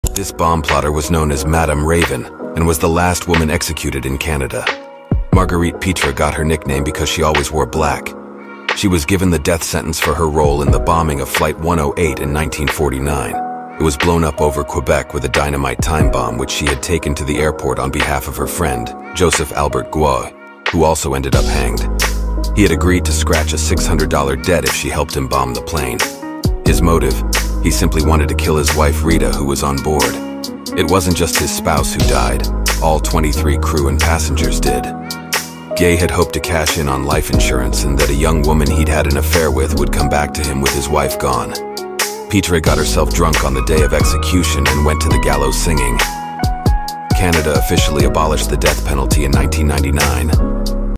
Madame Raven - the last woman executed by Canada
Canada only executed 13 women before abolishing capital punishment and the last was 'Madame Raven' aka Marguerite Pitre. She had been involved in horrific bomb plot in which whole plane was brought down just so her friend could kill his wife. This is her story in a short.

This channel will bring you unusual stories from history, often shocking, often surprising.